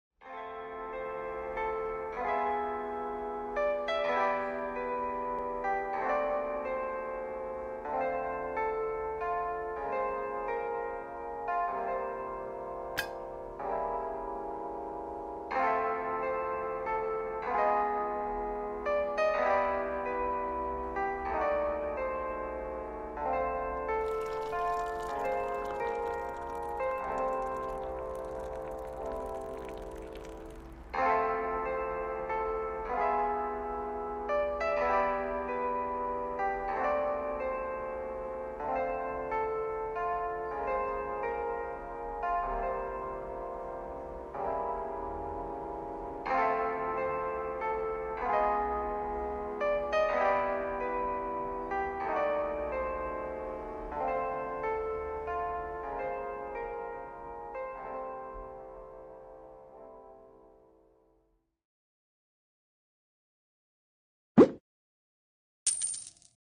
Paper Stop Motion - Having a meal with my own Anime Character
It was the first time I tried to make this kind of video，so close to the original characters that created by my own.